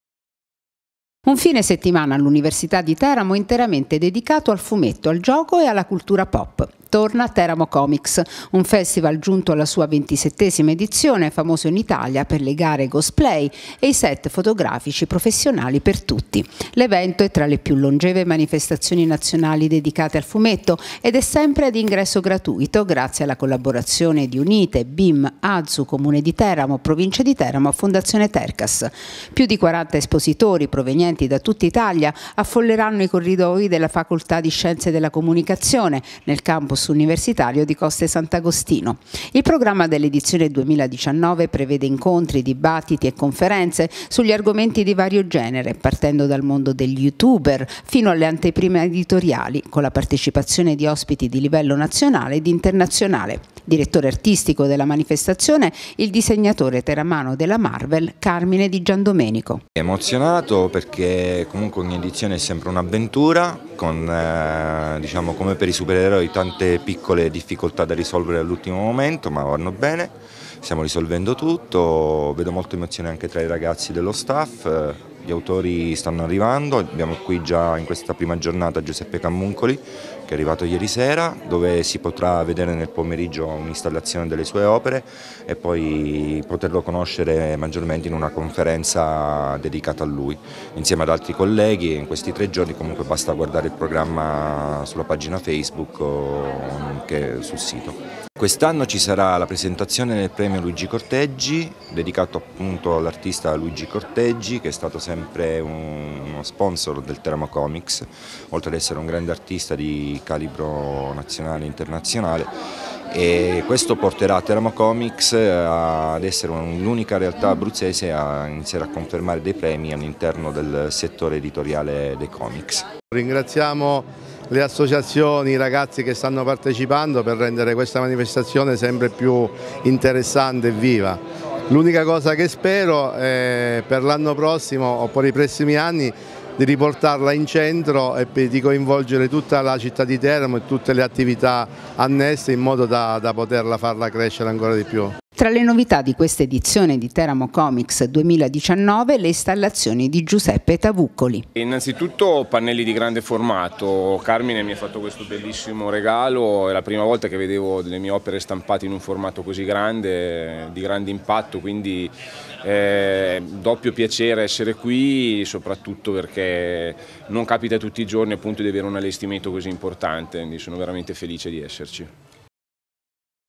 Teramo Comix: il festival del fumetto interattivo
Da oggi fino a domenica, il campus dell'Università di Teramo opsiterà la 27° edizione di Teramo Comix, il festival del Fumetto interattivo ormai famoso in Italia. L'evento attira migliaia di espositori, disegnatori e amanti del genere